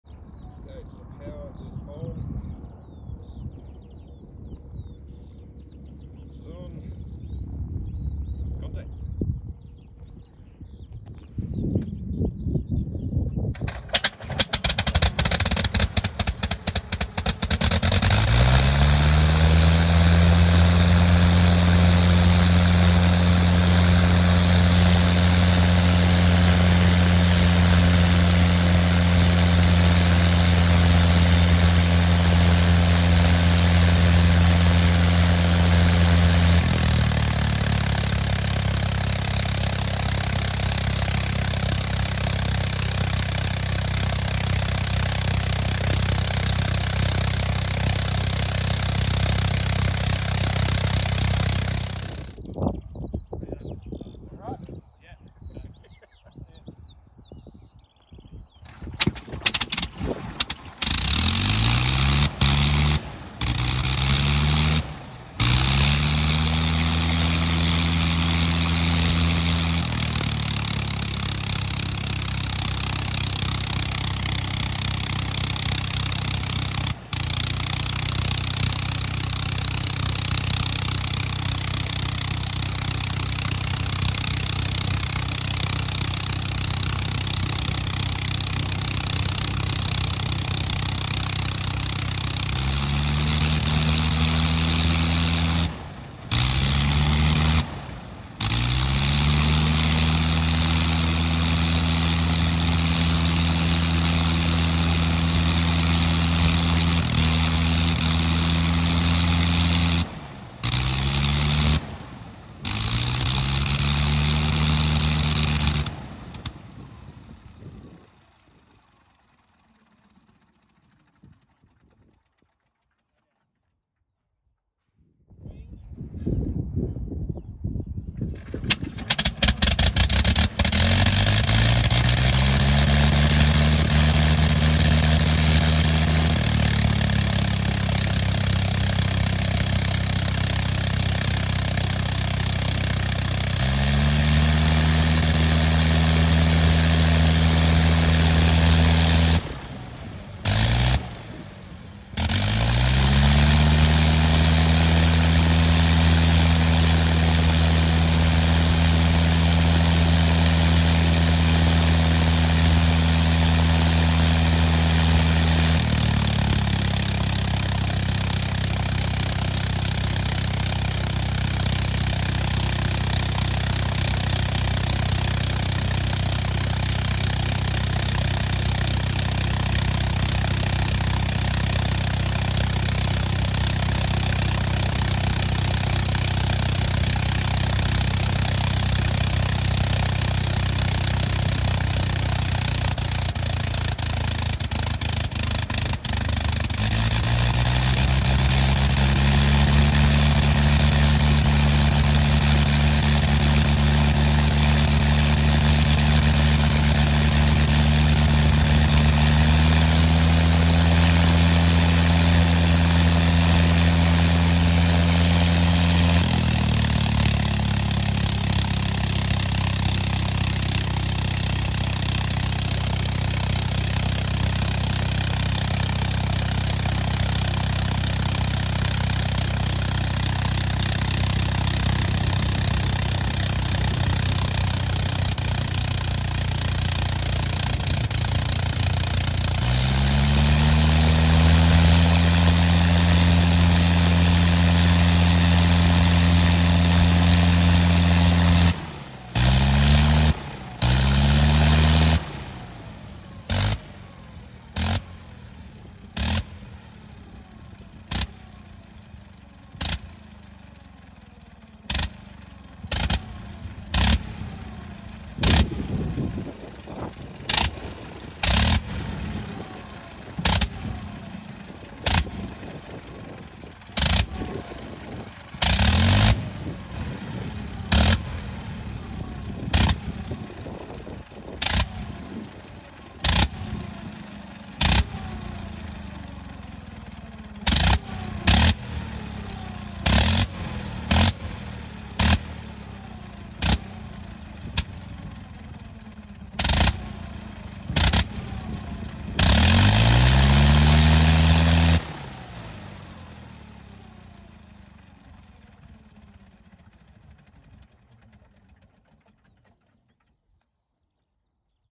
Fokker E.III Eindecker reproduction engine runs
This Eindecker (an Engels E.4 full-scale reproduction of the classic Great War fighter aircraft) is operated by the Omaka Aviation Heritage Centre in Blenheim, New Zealand on long term loan from builder Achim Engels of Germany. It is powered by a 9-cylinder Gnome Monosupape rotary engine built by Tony Wytenburg's Classic Aero Machining Services (also located at Omaka Aerodrome).

In this video we see Dan Coleman, Graham Orphan and Wayne Tantrum take turns running this magnificent machine on a lovely Marlborough evening (unfortunately I did not film Aaron Patchett's run). It is a sight, sound and smell that needs to be experienced in person to be fully appreciated!

This was filmed on an iPhone 11 and due to limitations of the camera the propeller and engine appear to be rotating in opposite directions: this is not the case on the actual machine, where the rotation of both is counter-clockwise when viewed from the cockpit.